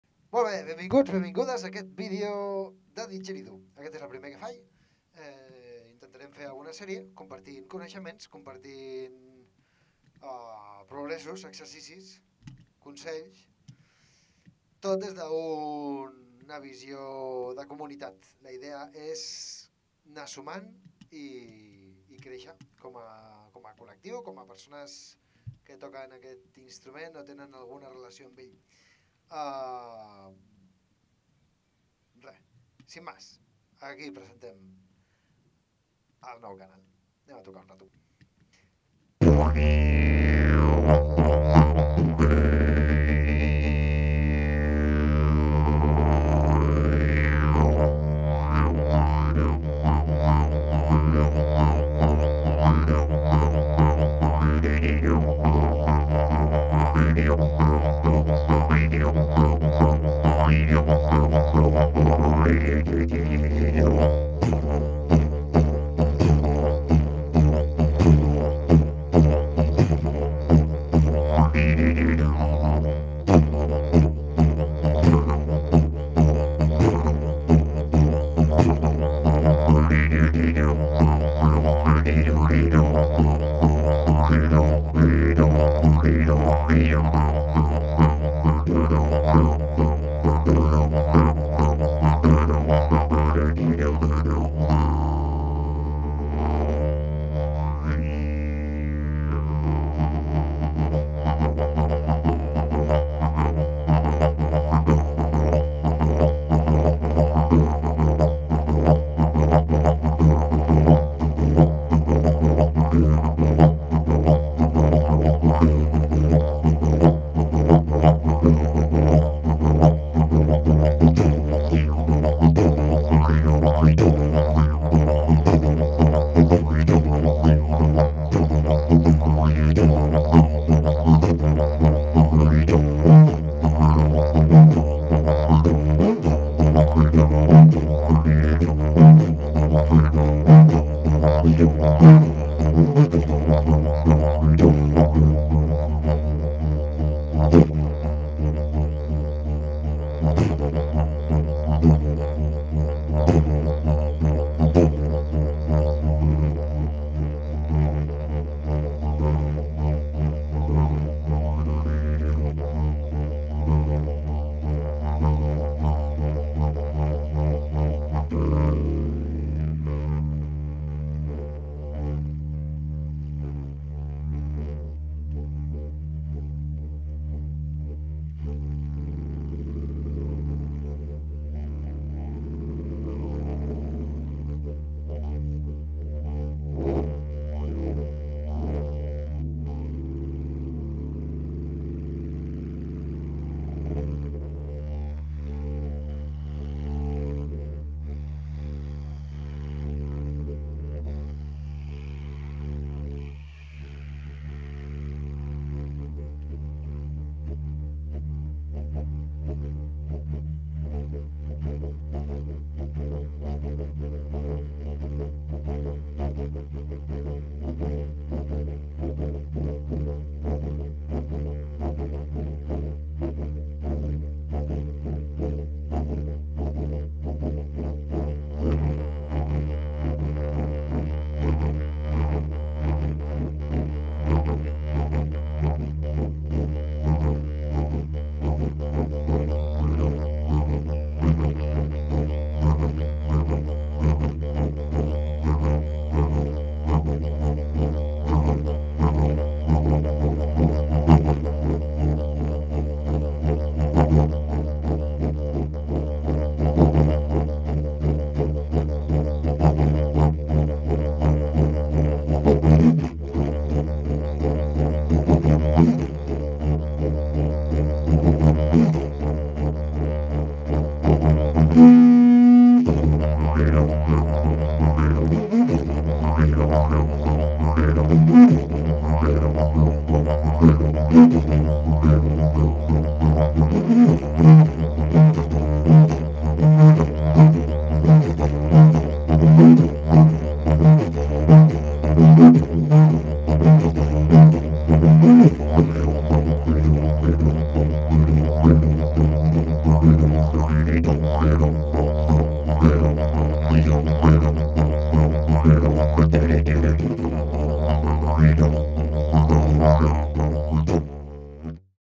One didge
Primer vídeo del canal. Espero amb aquest vídeo que si algú m'està veient amb un didge a prop, l'agafi i toqui una estona!

L'objectiu d'aquesta sèrie és compartir exercicis que jo mateix practico, per tant, no ensenyo res, sols comparteixo el meu progrés, per automotivar-me i motivar a altres.
Faré una petita secció tutorial, però crec que tutorials hi ha a puntades, oi?
Gràcies per ser-hi, Sergi.